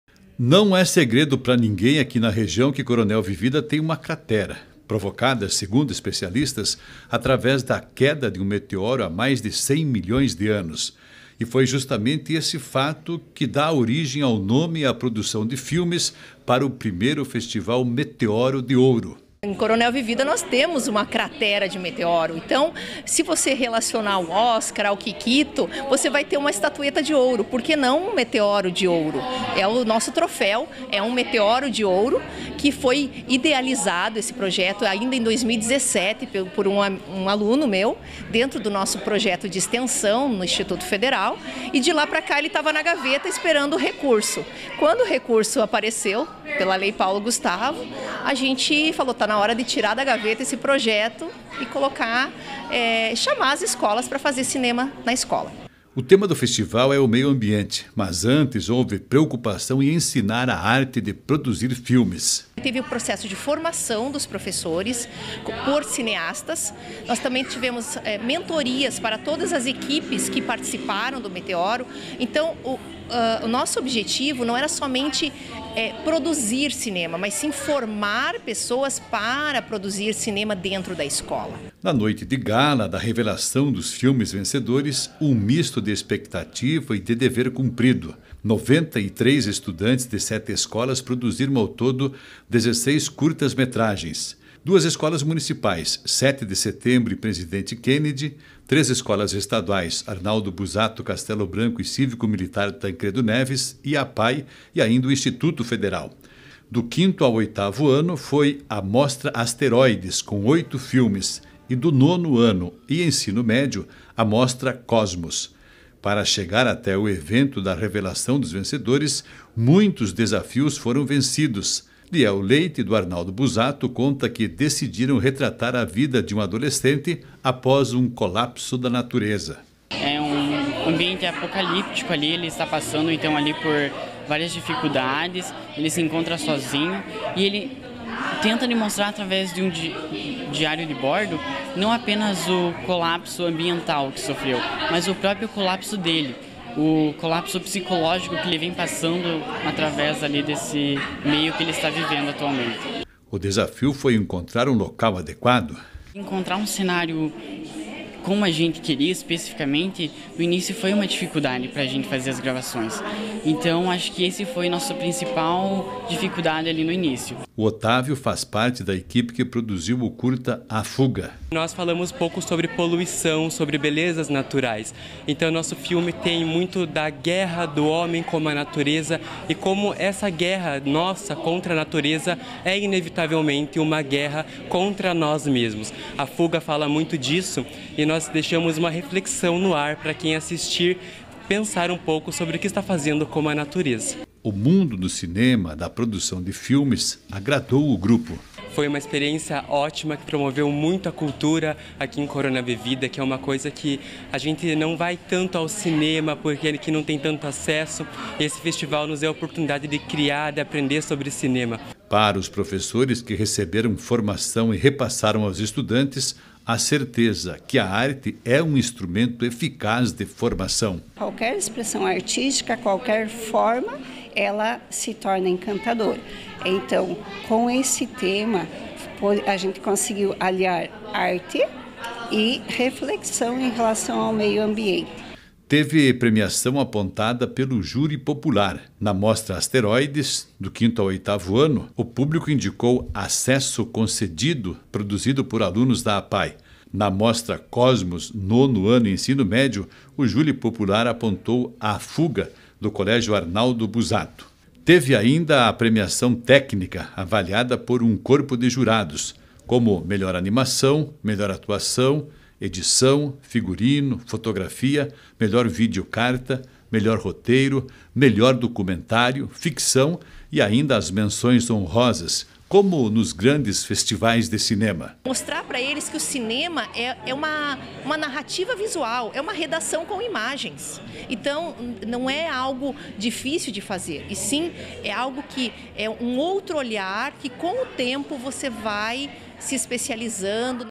Realizado em Coronel Vivida o primeiro Festival Meteoro de Curtas
Cultura: 16 filmes foram produzidos pelas escolas do município.